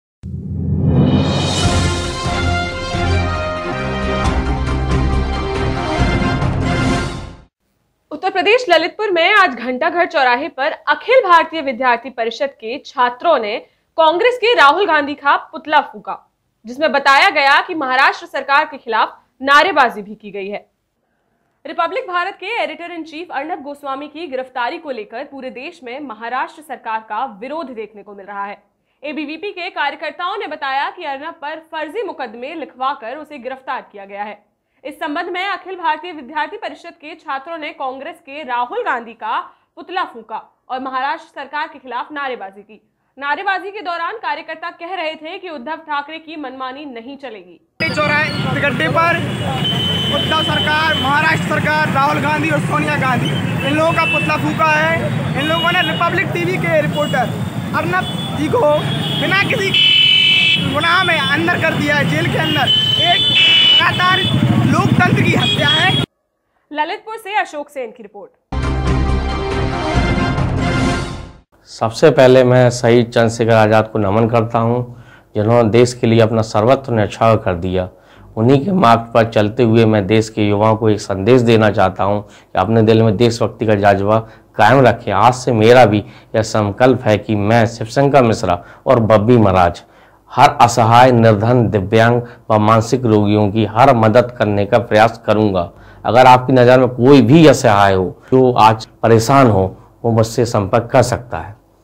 ललितपुर में अलग-अलग सड़क दुर्घटनाओं में एक की मौत 6 घायल।
Report : Ashok Sen